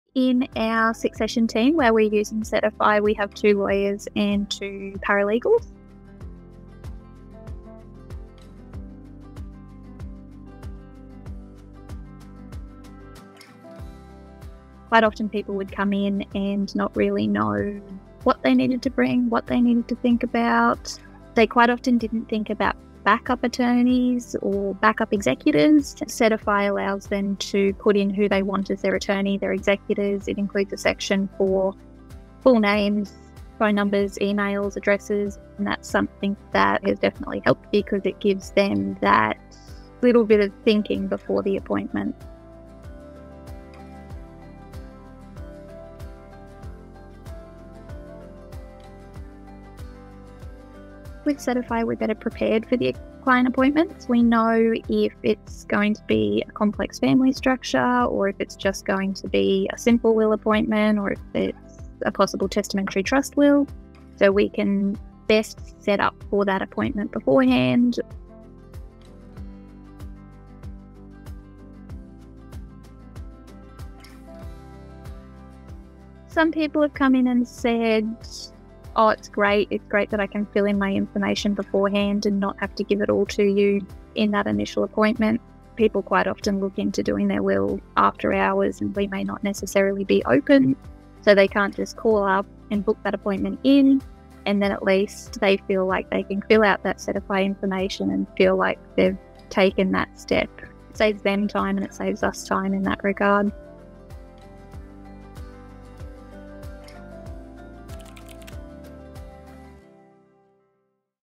Chloe Whereat Client Testimonial
Chloe Whereat (Associate Lawyer in the Succession Team), Bradley + Bray discusses his experience using Settify - the award-winning client onbaording platform for law firms. Chloe speaks about the benefits and advantages of using Settify for her clients and staff.